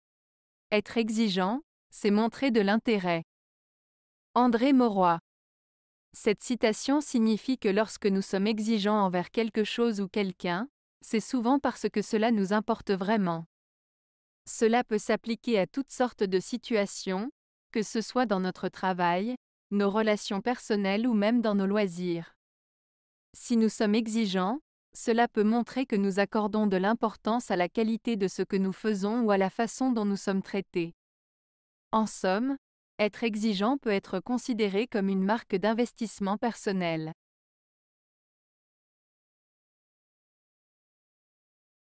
Être exigeant, c'est montrer de l'intérêt. - André Maurois
Cette citation signifie que lorsque nous sommes exigeants envers quelque chose ou quelqu'un, c'est souvent parce que cela nous importe vraiment. Cela peut s'appliquer à toutes sortes de situations, que ce soit dans notre travail, nos relations personnelles ou même dans nos loisirs. Si nous sommes exigeants, cela peut montrer que nous accordons de l'importance à la qualité de ce que nous faisons ou à la façon dont nous sommes traités. En somme, être exigeant peut être considéré comme une marque d'investissement personnel.